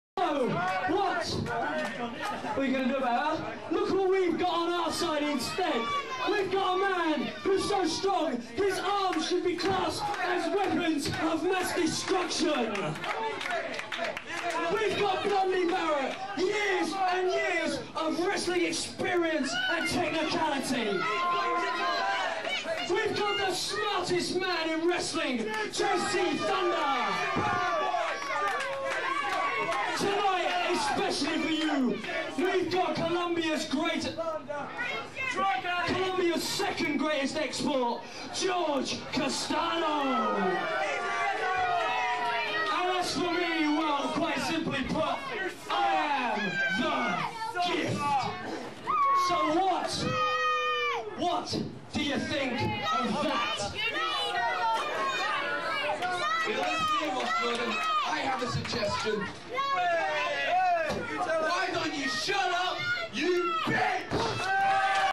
Ross Jordan introduces the Southern Alliance in Nottingham (April 2004) - RBW Wrestling
Ross Jordan introduces the Southern Alliance in Nottingham (April 2004) - Revolution British Wrestling